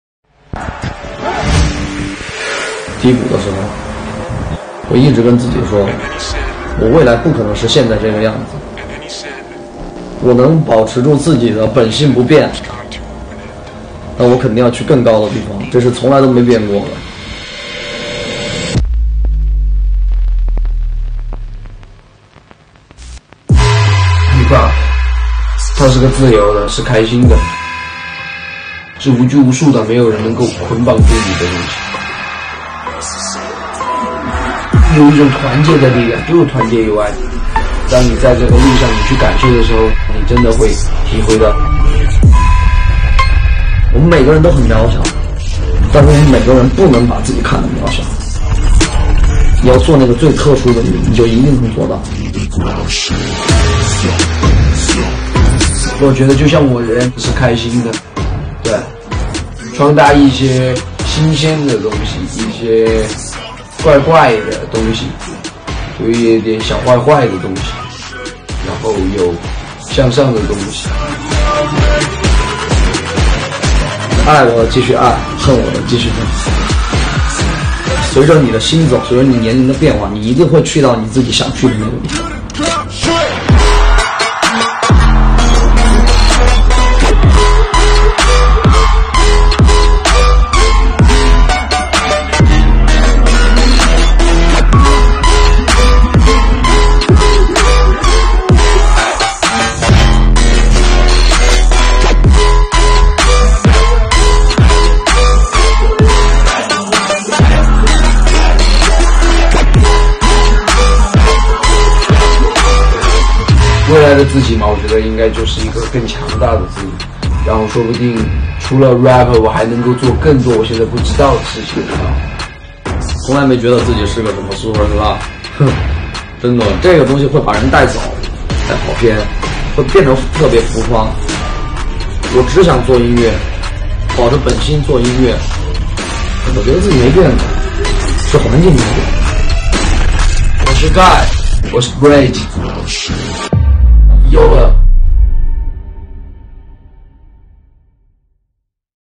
Bridge布瑞吉‘HIPHOP有一种团结的力量‘’/GAI‘恨我的继续很’/YOLO 演出片段剪辑/GOSH/ SUP
Bridge：“HIPHOP有一种团结的力量，就是团结友爱，当你在这个路上去感受的时候，你真的会体会到。”
GAI：“爱我的继续爱，恨我的继续恨。”